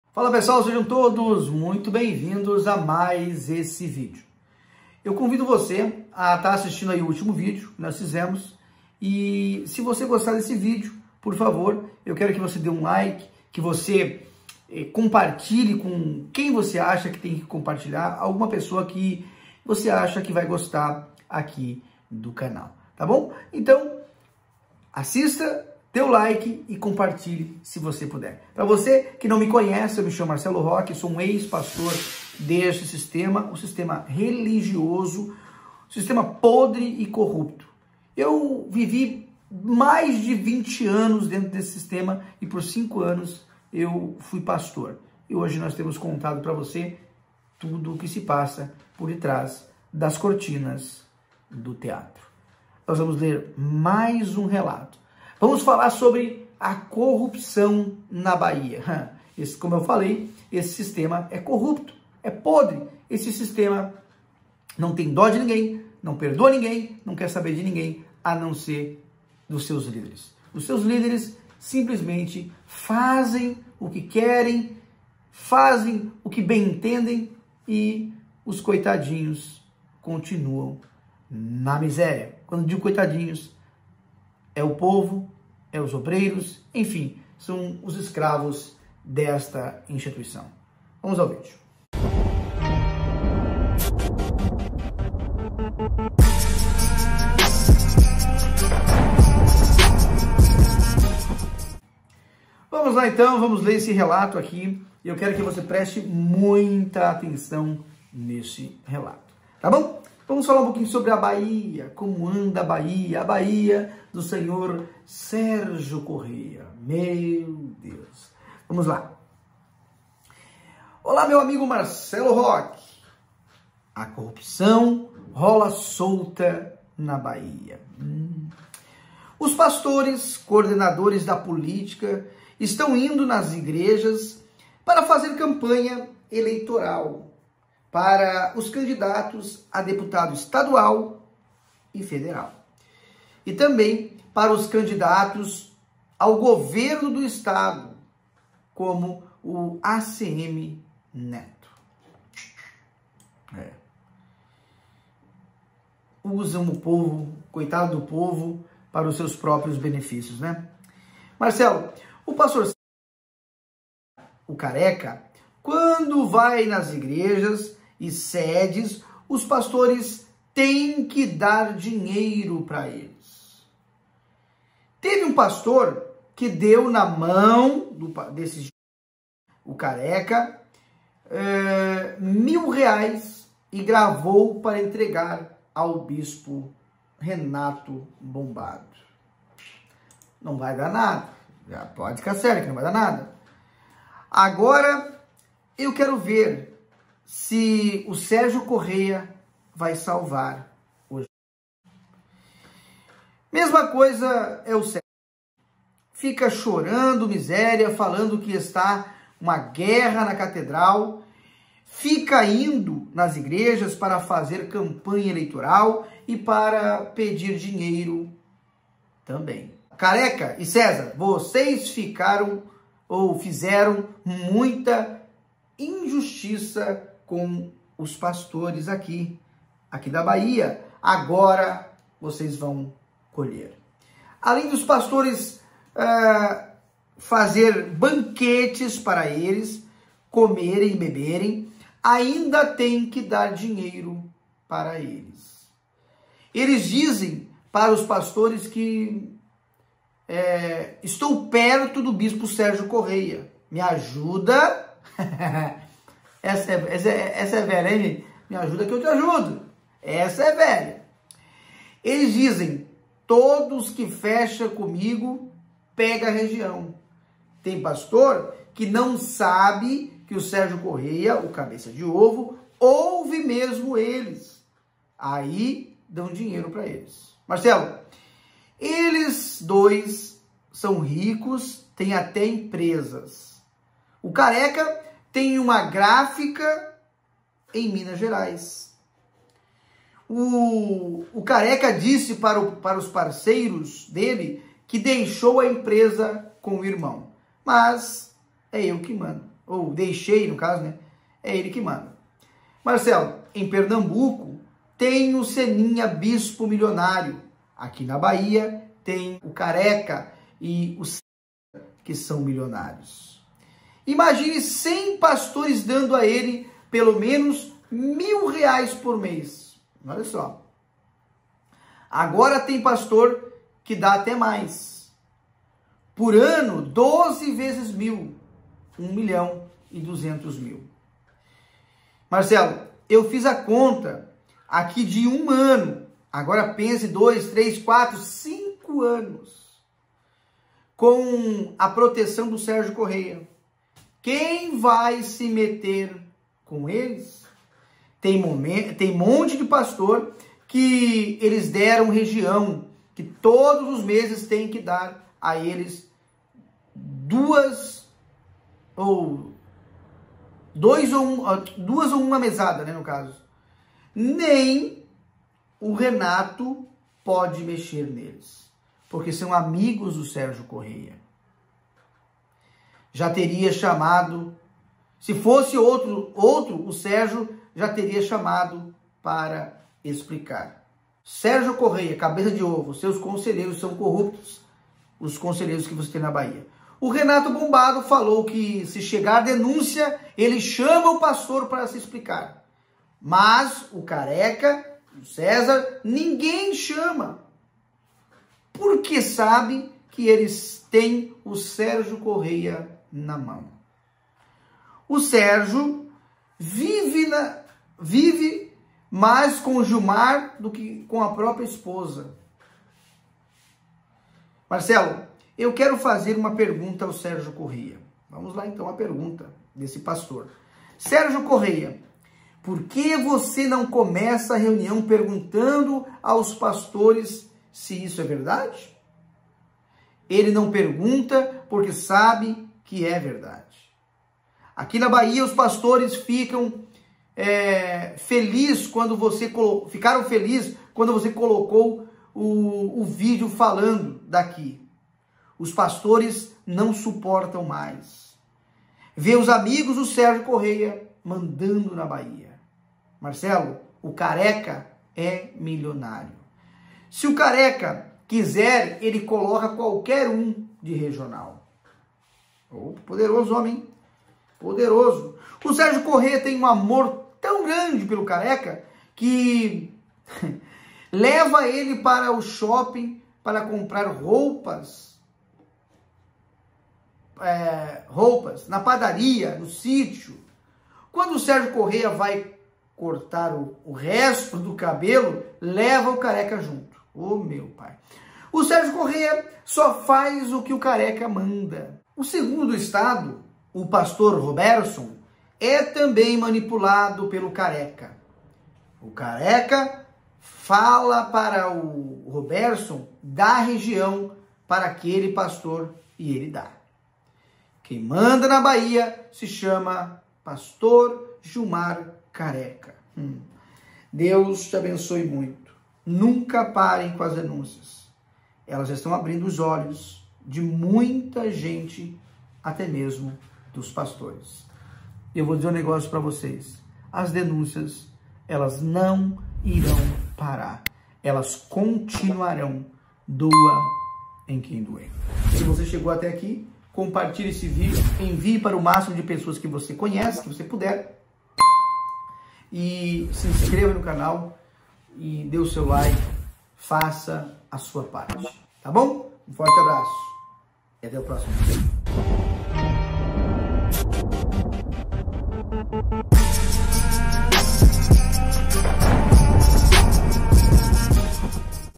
CORRUPÇÃO ROLA SOLTA NA BAHIA! SERGIO CORRÊA SABE DE TUDO
Olá meu amigo Marcelo Roque!

CORRUPÇÃO ROLA SOLTA AQUI NA BAHIA!

OS PASTORES COORDENADORES DA POLÍTICA ESTÃO INDO NAS IGREJAS PARA FAZER CAMPANHA ELEITORAL PARA OS CANDIDATOS A DEPUTADO ESTADUAL E FEDERAL.

E TAMBÉM PARA O CANDIDATO AO GOVERNO DO ESTADO ACM NETO.

MARCELO, O PR.CÉZAR E GILMAR CARECA QUANDO VAI NAS IGREJAS E SEDES OS PASTORES TEM QUE DAR DINHEIRO PARA ELES.

TEVE UM PASTOR QUE DEU NA MÃO DO GILMAR CARECA 1 MIL REAIS  E GRAVOU PARA ENTREGA AO BP.RENATO ( BOMBADO )

AGORA EU QUERO VER SE SÉRGIO CORREIA VAI SALVA O GILMAR!

MESMA COISA O PR.CÉZAR FICA CHORANDO MISÉRIA FALANDO QUE ESTA UMA GUERRA  CATEDRAL, FICA INDO NAS IGREJAS PARA FAZER CAMPANHA ELEITORAL E PARA PEDI DINHEIRO TAMBÉM.

GILMAR CARECA E CÉZAR VOCÊS FIZERAM MUITA INJUSTIÇA COM OS PASTORES AQUI DA BAHIA AGORA VAI COLHER!!!

ALÉM DOS PASTORES FAZEREM BANQUETES PARA ELES COMEREM BEBEREM, AINDA TEM QUE DAR DINHEIRO PARA ELES.

ELES DIZEM PARA OS PASTORES EU ESTOU PERTO DO BISPO SÉRGIO CORREIA, ME AJUDA QUE TE AJUDO.

ELES DIZEM TODOS QUE FECHA COMIGO PEGA REGIÃO, TEM PASTOR QUE SABE QUE O SÉRGIO CORREIA (CABEÇA DE OVO) OUVE MESMO ELES AÍ DÃO DINHEIRO PARA ELES.

MARCELO, ELES DOIS SÃO RICOS TEM ATÉ EMPRESAS.

ESSE GILMAR CARECA TEM UMA GRÁFICA EM MINAS GERAIS.

O GILMAR CARECA DISSI PARA OS PARCEIROS DELE QUE DEIXOU A EMPRESA COM IRMÃO, MAS É EU QUE MANDO.

MARCELO, EM PERNAMBUCO TEM O SENINHA BISPO MILIONÁRIO, AQUI NA BAHIA TEM O PR.GILMAR CARECA E O PR.CÉZAR QUE SÃO MILIONÁRIOS.

IMAGINE 100 PASTORES DANDO A ELE PELO MENOS 1 MIL REAIS POR MÊS.

AGORA TEM PASTORES QUE DAR ATÉ MAS.

POR ANO 12 X 100 MIL = 1 MILHÃO E 200 MIL

MARCELO, EU FIZ CONTA AQUI DE UM ANO AGORA PENSE 2,3,4,5, ANOS

COM A PROTEÇÃO DO SÉRGIO CORREIA  QUEM VAI SE METE COM ELES.

TEM MONTE DE PASTORES QUE ELES DERAM REGIÃO QUE TODOS OS MESES TEM QUE DAR A ELES DOIS UMA MESADA.

NEM O BP.RENATO PODE MEXER NELES.

PORQUE SÃO AMIGOS DO SÉRGIO CORREIA DE LONGAS DATAS.

SE FOSSE OUTROS O SÉRGIO CORREIA JA TERIA CHAMADO PARA SE EXPLICAR.

SERGIO CORREIA (CABEÇA DE OVO)

SEUS CONSELHEIROS SÃO MAS CORRUPTO QUE TEM NA BAHIA.

O RENATO BOMBADO FALOU QUE SE CHEGA DENÚNCIA ELE CHAMA O PASTOR PARA SE EXPLICAR.

MAS O GILMAR CARECA E CÉZAR NINGUÉM CHAMA POR QUE SABE QUE ELES TÊM SÉRGIO CORREIA NA MÃO.

O SÉRGIO CORREIA VIVE MAS COM GILMAR DO QUE A PRÓPRIA ESPOSA DO GILMAR.

MARCELO, EU QUERO FAZER UMA PERGUNTA AO SÉRGIO CORREIA.

SÉRGIO CORREIA POR QUE VOCÊ NÃO COMEÇA A REUNIÃO PERGUNTANDO AOS PASTORES SE ISSO EVERDADE ???

ELE NÃO PERGUNTA PORQUE SABE QUE É VERDADE.

Aqui na Bahia os pastores fica feliz quando você colocou vídeos falando daqui.

OS PASTORES NÃO SUPORTA MAIS, VÊ OS AMIGOS DO SÉRGIO CORREIA MANDANDO NA BAHIA.

MARCELO, O GILMAR CARECA É  MILIONÁRIOS.

SE O GILMAR CARECA QUISER ELE COLOCA QUALQUER UM DE REGIONAL.

O SÉRGIO CORREIA TEM UM AMOR TÃO GRANDE PELO GILMAR QUE LEVA ELE PARA O SHOPPING PARA COMPRAR ROUPAS,PADARIA,SÍTIO.

QUANDO O SÉRGIO CORREIA VAI CORTA O RESTO DO CABELO LEVA O GILMAR CARECA.

O SÉRGIO CORREIA SÓ FAZ O QUE O GILMAR MANDA.

O SEGUNDO DO ESTADO PR.ROBERSON É TAMBÉM MANIPULADO PELO GILMAR CARECA.

SE O GILMAR CARECA FALAR PARA O PR.BROBERSON DÁ REGIÃO PARA AQUELE PASTOR  ELE DÁ.

QUEM MANDA NA BAHIA SE CHAMA PASTOR GILMAR CARECA.

Deus te abençoe muito!

Nunca pare as denúncias elas estão abrindo olhos de muita gente até dos pastores!

SE VOCE QUISER FAZER PARTE DO PROJETO QUE VAI AJUDAR PESSOAS CARENTES, ENTRE NESTE GRUPO DO TELEGRAM👇

REDE SOCIAIS!